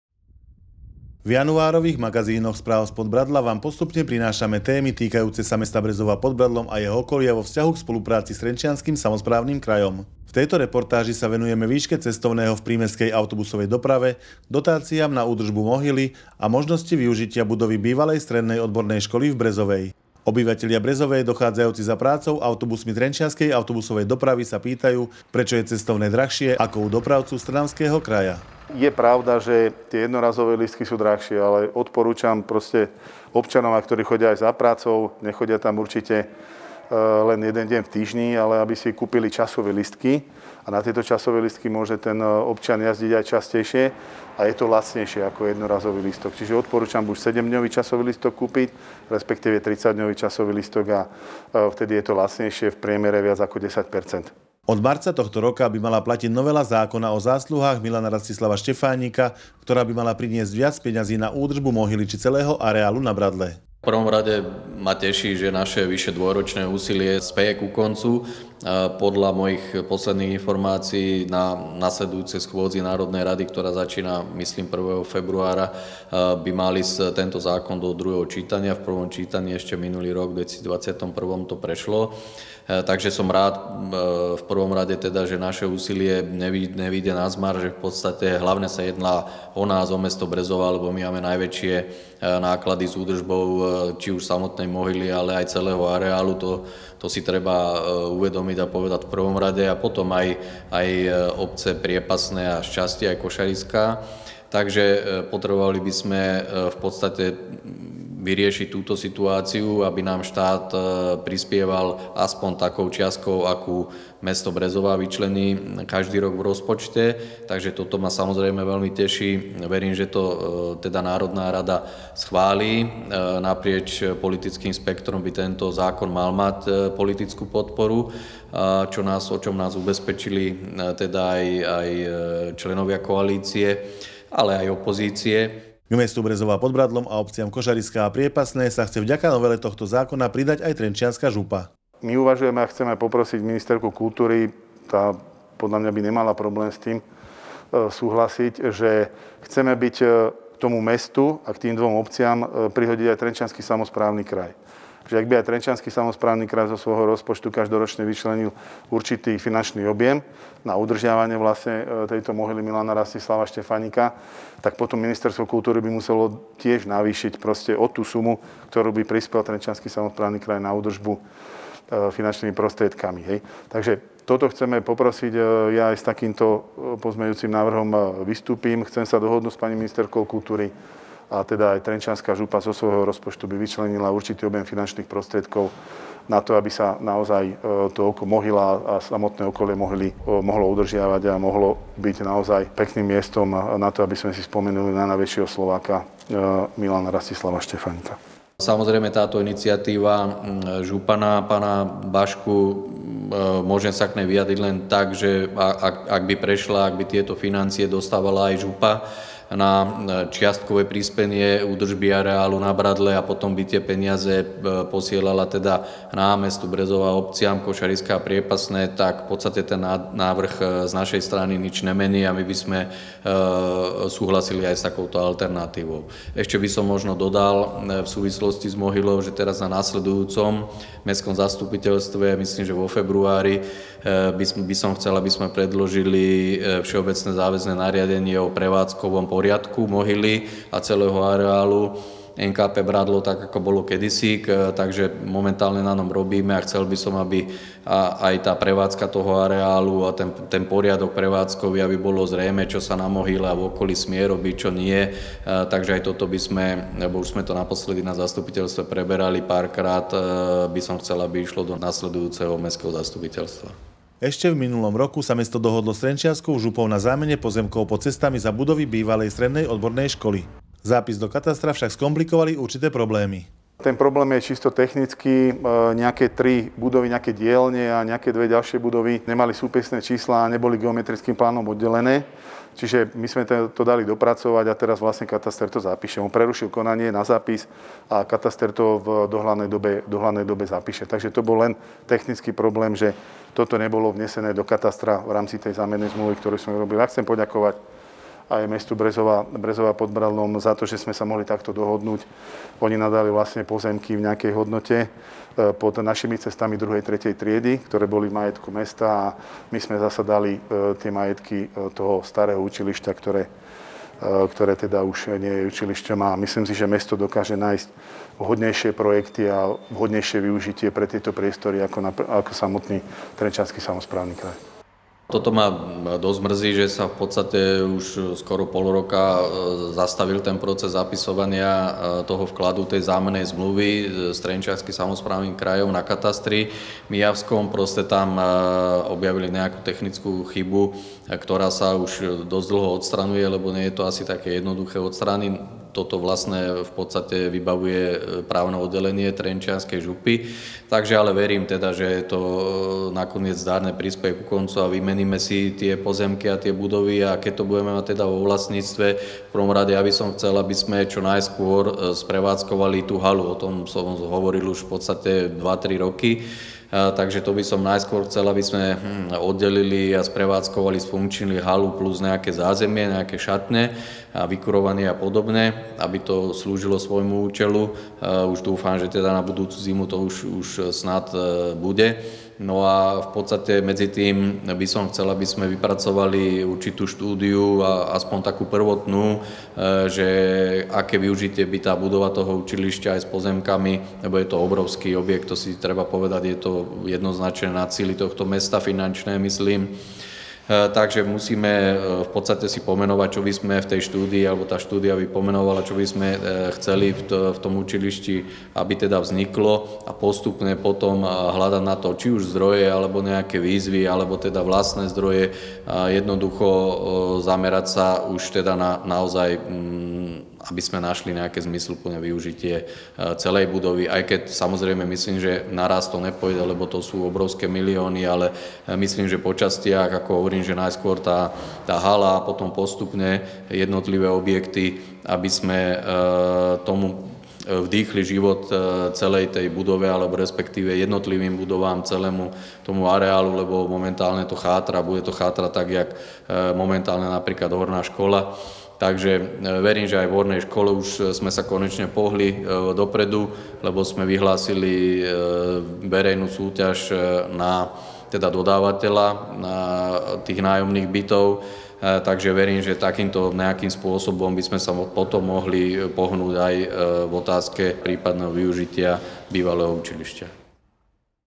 Na údržbe Mohyly sa okrem mesta Brezová chce podieľať aj Trenčiansky samosprávny kraj
V januárových magazínoch Správ spod Bradla Vám postupne prinášame témy týkajúce sa mesta Brezová pod Bradlom a jeho okolia vo vzťahu k spolupráci s Trenčianskym samosprávnym krajom. V tejto reportáži sa venujeme výške cestovného v prímestskej autobusovej doprave, dotáciám na údržbu Mohyly a možnosti využitia budovy bývalej strednej odbornej školy v Brezovej. Obyvatelia Brezovej dochádzajúci za prácou autobusmi Trenčianskej autobusovej dopravy sa pýtajú, prečo je cestovné drahšie, ako u dopravcu z Trnavského kraja. Od marca tohto roka by mala platiť novela Zákona o zásluhách Milana Rastislava Štefánika, ktorá by mala priniesť viac peňazí na údržbu Mohyly či celého areálu na Bradle. K mestu Brezová pod Bradlom a obciam Košariská a Priepasné sa chce vďaka novele tohto zákona pridať aj Trenčianska župa. Ešte v minulom roku sa mesto dohodlo s trenčianskou župou na zámene pozemkov pod cestami za budovy bývalej SOŠ. Zápis do katastra však skomplikovali určité problémy. Zdroj: TV Sen,
Redaktor: Andrej Hacaj , Kamera: Andrej Hacaj